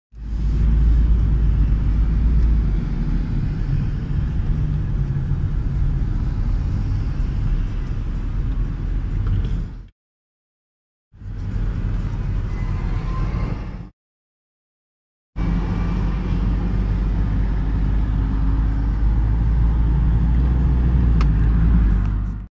Страшные кадры протараненных байкеров
Два УАЗа протаранили колонну байкеров. Беларусь  июнь 2017